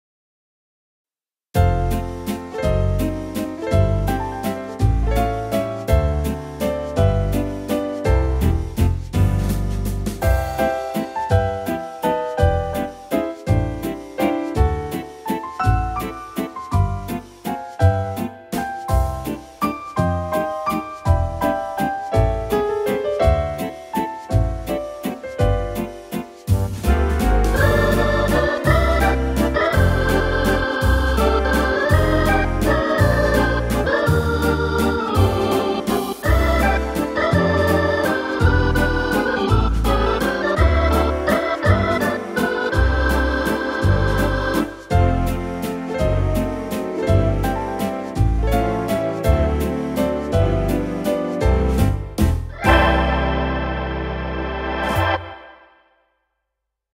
Sing Along Waltz
Demo of the Premium Style by the Yamaha Tyros4. in March 2011